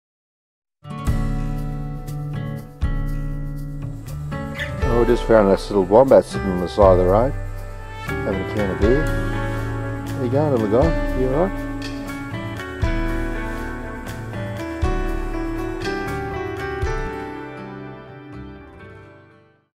BABY AUSTRALIAN WOMBAT DRUNK ON BEER ?
This absolutely gorgeous baby wombat a native marsupial of Australia was found beside the road with a can of beer. He did seem a bit wobbly, and i began to wonder if he had been hit by a car. Then he took of at a wombat run and i guess he was fine or was just going home after a fun day out roaming, drinking and generally just being an adorable wombat.